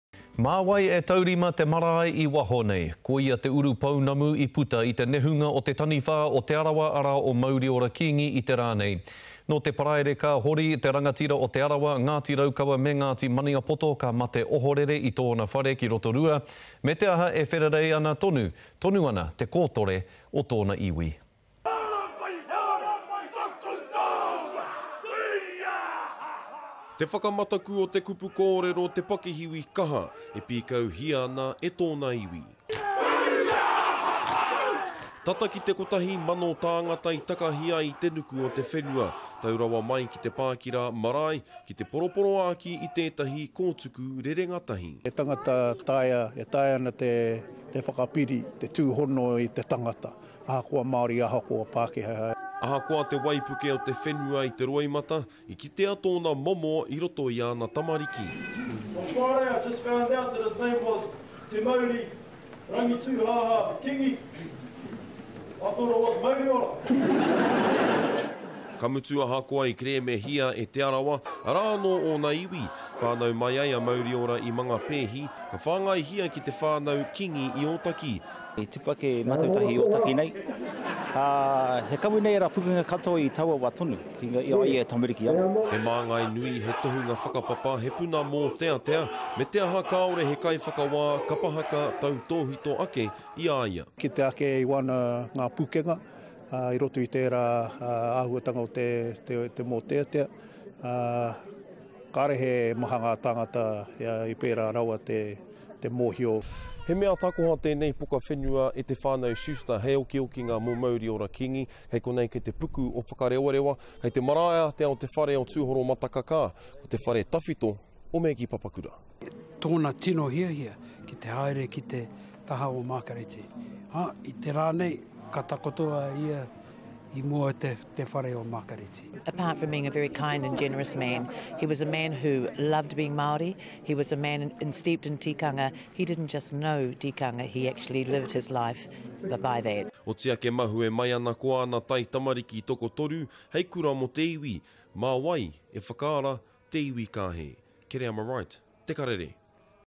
Te Arawa great, Mauriora Kingi laid to rest
Who will tend to the needs of the marae and iwi? That’s the question pondered on today as Mauriora Kingi, one of Te Arawa’s sons was laid to rest. With whakapapa links to Ngāti Maniapoto and Ngāti Raukawa, his people continue to mourn for him. He suddenly passed away last Friday in his Rotorua home.